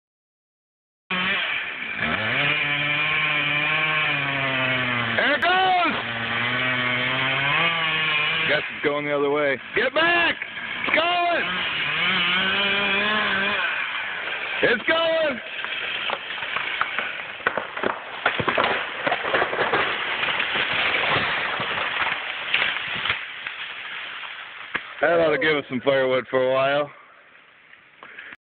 Kenny C finally fells the 100' tall standing dead oak, vid 3 of 3.
After making the face cut and the realizing the tree is going to fall in the opposite direction he was intending it to, Kenny drops the monster standing dead red oak tree.  If you haven't already, be sure to like Joe's Premium Firewood on Facebook.